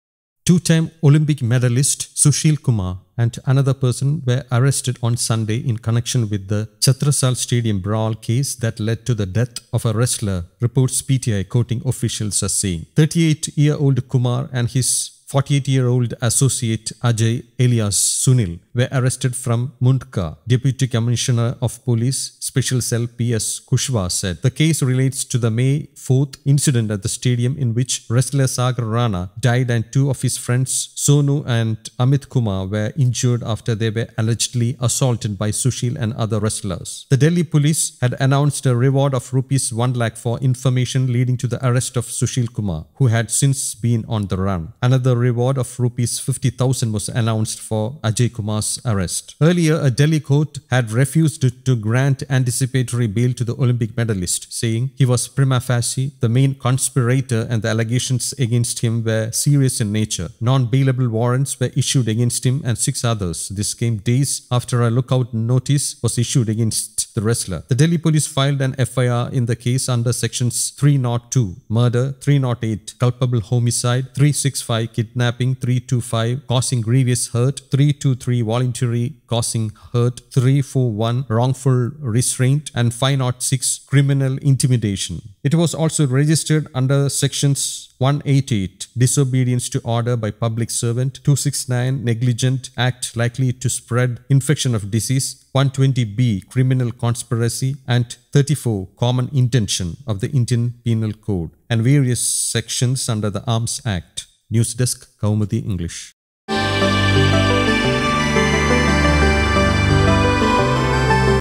Two-time Olympic medallist Sushil Kumar arrested in wrestler murder case
Two-time Olympic medallist Sushil Kumar and another person were arrested on Sunday in connection with the Chhatrasal Stadium brawl case that led to the death of a wrestler here, reports PTI quoting officials as saying.
Kumar (38) and his associate Ajay, alias Sunil, (48) were arrested from Mundka, Deputy Commissioner of Police (Special Cell) P S Kushwah said.
The case relates to the May 4 incident at the stadium in which wrestler Sagar Rana died and two of his friends, Sonu and Amit Kumar, were injured after they were allegedly assaulted by Sushil and other wrestlers.
The Delhi Police had announced a reward of Rs one lakh for information leading to the arrest of Sushil Kumar, who had since been on the run.

Another reward of Rs 50,000 was announced for Ajay Kumar's arrest.
Earlier, a Delhi Court had refused to grant anticipatory bail to the Olympic medallist, saying he was prima facie the main conspirator and allegations against him were serious in nature.
Non-bailable warrants were issued against him and six others. This came days after a lookout notice was issued against the wrestler.